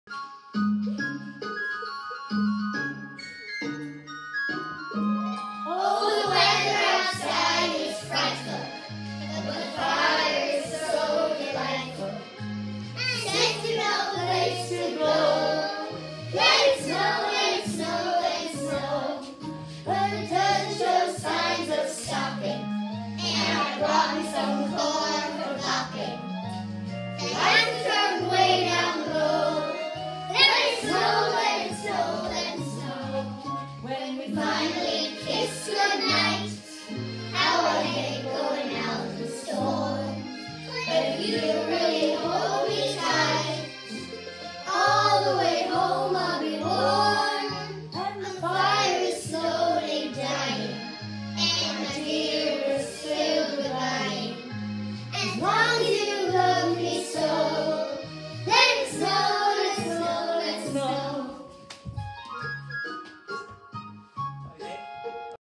Glenroe Community National School, Kilmallock, Co. Limerick- Christmas Carol School Competition 2021
Glenroe Community National School in Glenroe, Kilmallock, Co. Limerick's entry to the Audiology Medical Services Christmas Carol School Competition 2021.